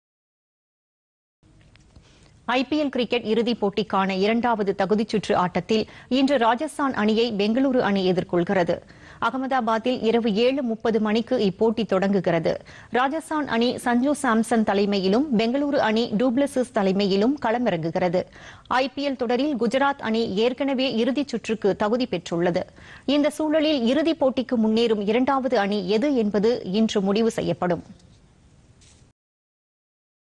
ஐபிஎல் கிரிக்கெட்PodhigaiTamilNews
ஐபிஎல் கிரிக்கெட்  -  இறுதிப் போட்டிக்கான இரண்டாவது தகுதி சுற்று ஆட்டத்தில் ராஜஸ்தான் - பெங்களூரு அணிகள் இன்று பலப்பரீட்சை